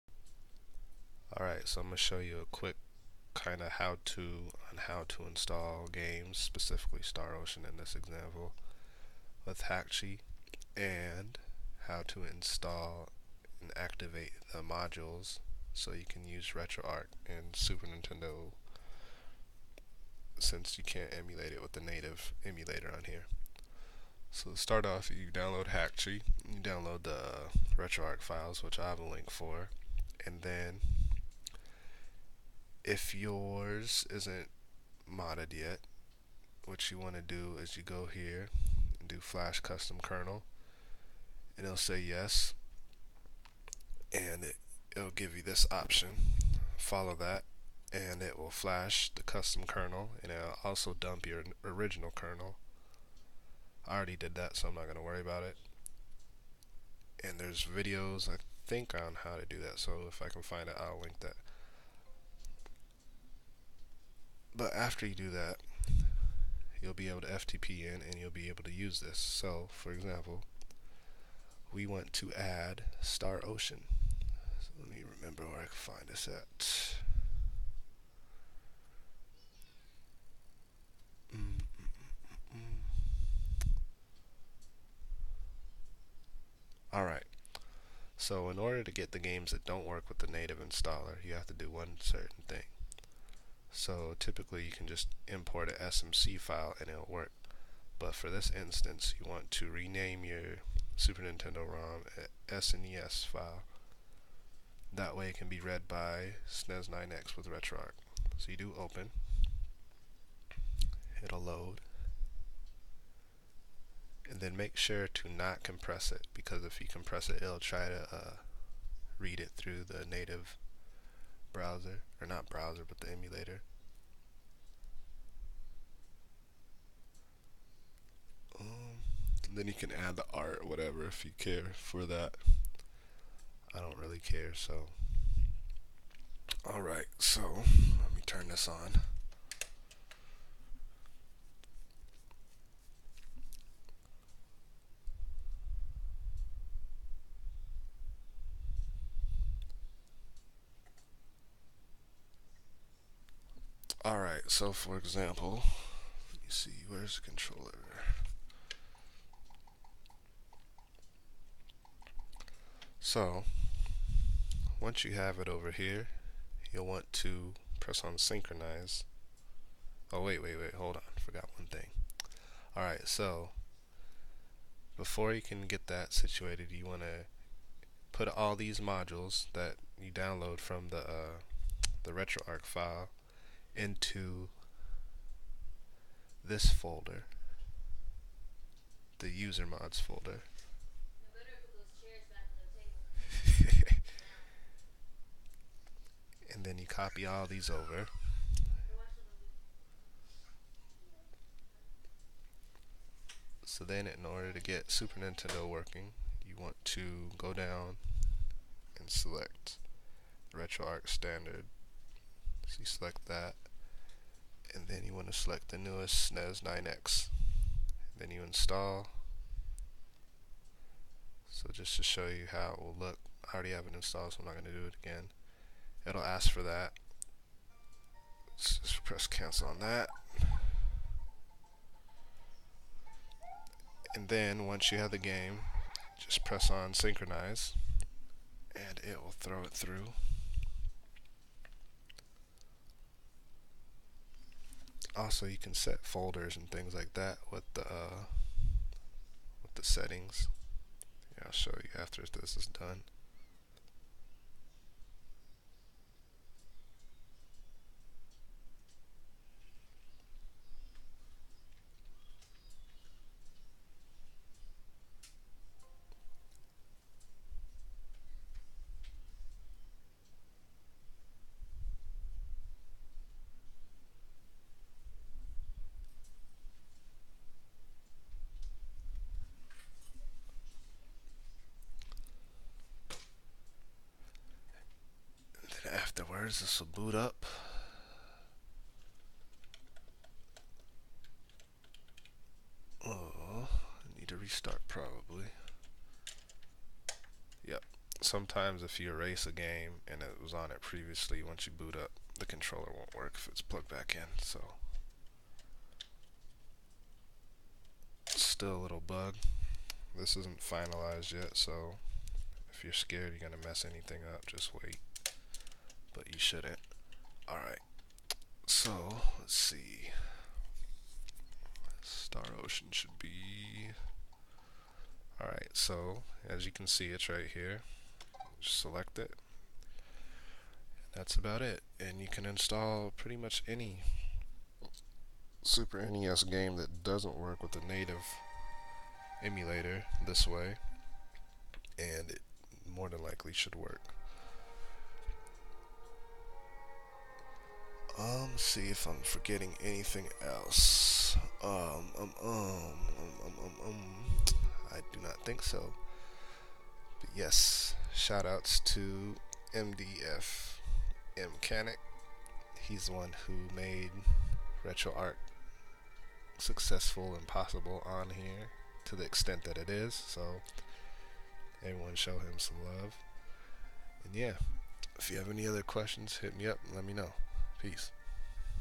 How to use SNES games that don't work with the Native Emulator in Hakchi
Thanks to everyone that has helped along the way! & special thanks to MDFMKanic for making these amazing Retroarch cores.

I suck at explaining things so I'll link you guys to more info in the comments.

DO THIS AT YOUR OWN RISK I DONT WANNA HEAR IT IF YOU BRICK SOMETHING! (I doubt you will though, just sayin ;P )